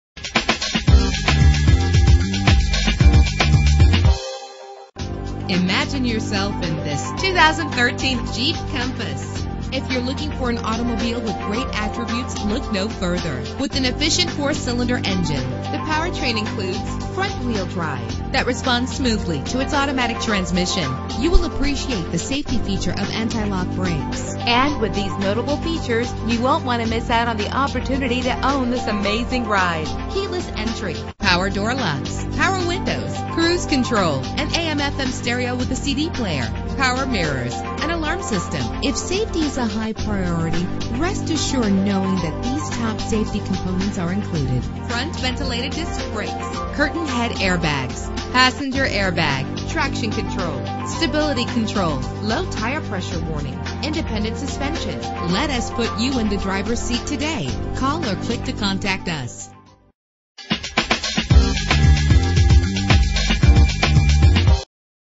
2013 Jeep Compass - Orland Park IL
Year: 2013
Make: Jeep
Model: Compass
Trim: Latitude
Engine: 2.0 liter inline 4 cylinder 16 valve
Transmission: Automatic CVT
Color: True Blue Pearlcoat
Mileage: 5
Address:
15941 S. 94th Ave.